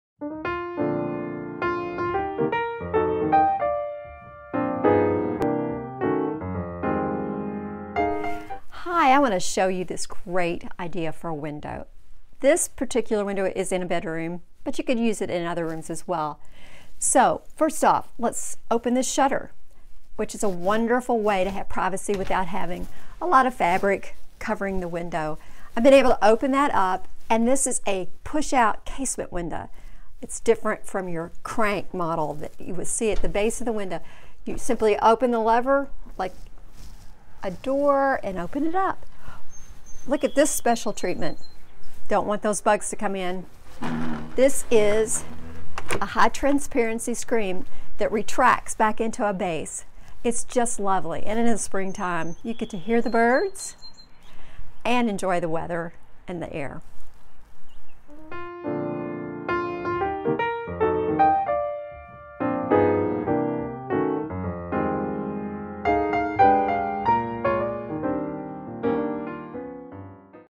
One Window, Many Design Ideas
Do you have a hard time opening your windows? Worried about bugs but think screens are plain ugly? Check out this solution for easily opening your windows with a hidden screen...and a great window treatment option without tons of heavy fabric! So many great design options in one window!